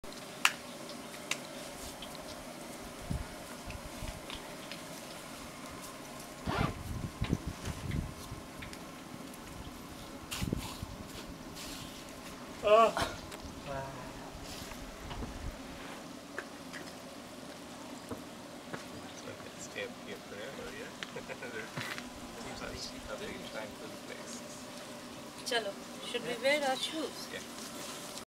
Outside Siddara Betta cave
My travel party enjoying the view with the site manager and some others near the entrance of Siddara Betta.